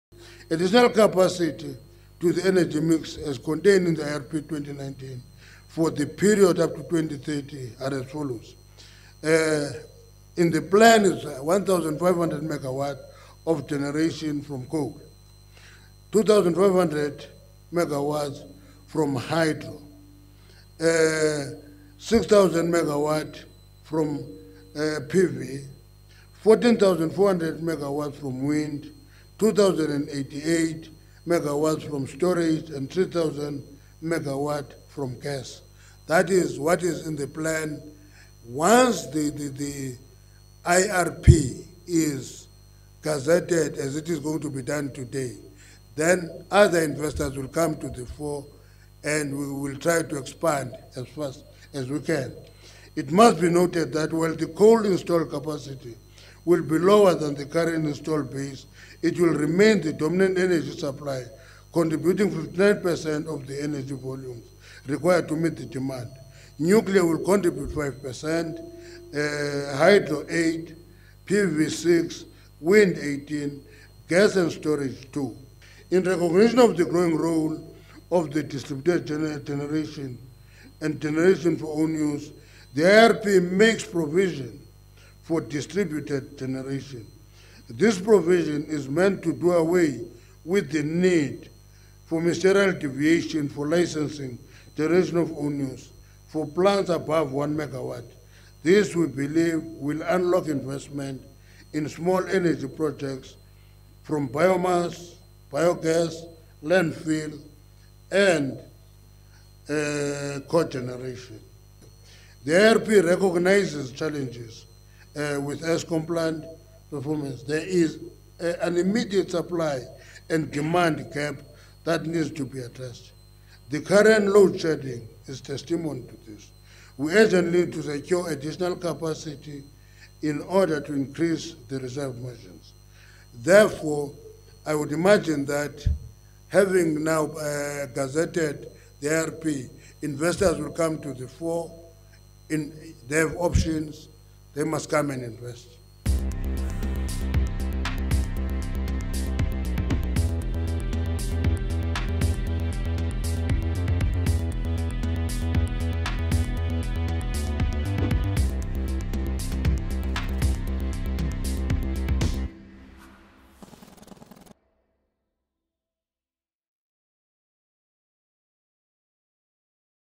South Africa to test market for emergency power ‘gap fillers’ as IRP is finalised
South Africa will release a request for information (RFI) in the coming weeks to test the market for solutions to close an immediate electricity supply gap of between 2 000 MW and 3 000 MW, which has emerged primarily as a result of a fall in the energy availability factor (EAF) from Eskom’s coal fleet and a de-rating of the anticipated output from the much-delayed Medupi and Kusile plants.